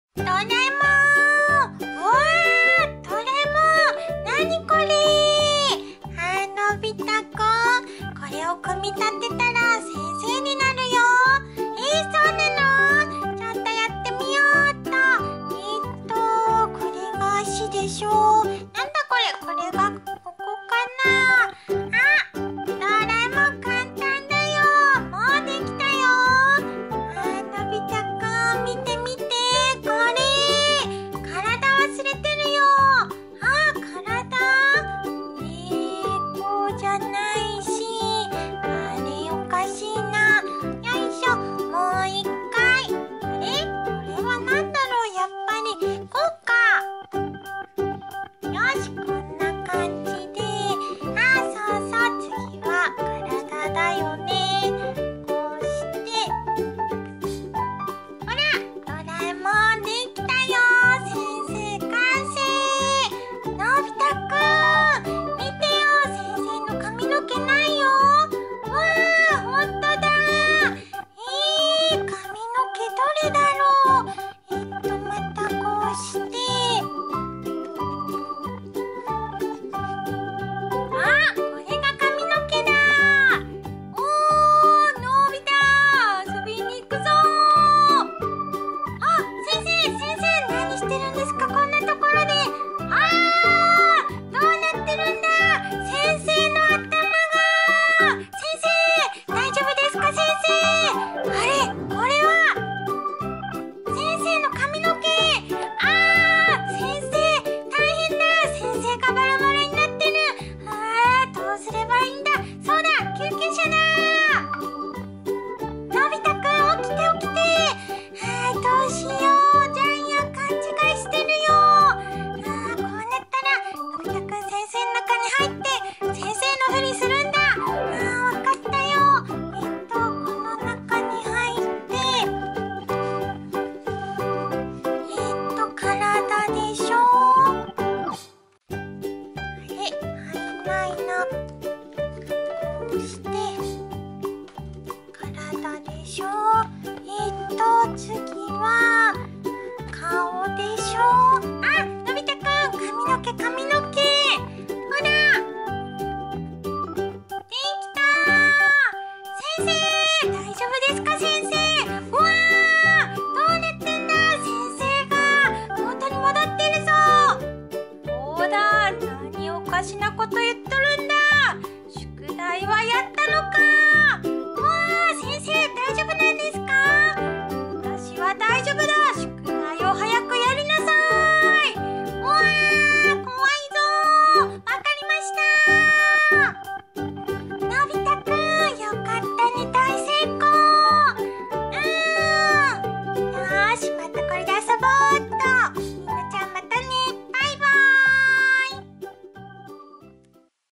ドラえもんとのび太君が先生の顔パーツと体パーツを組み立てて先生を作るよ！先生が変だよ！のび太の部屋
ドラえもんとのび太君がばらばらになった先生の顔パーツと体パーツを組み立てて先生を作るけど変な先生になっちゃうよ！のび太君が先生の中に入ってジャイアンを追い出すよ！
ドラえもんが先生ロボを押し入れに隠して昼寝するのび太君に宿題をさせるよ！先生の顔と体を組み立てるよ！のび太の部屋
ドラえもんが先生の顔と体パーツでのび太君を先生にしてジャイアンを追いだすよ！先生の顔の向きが変だよ！のび太の部屋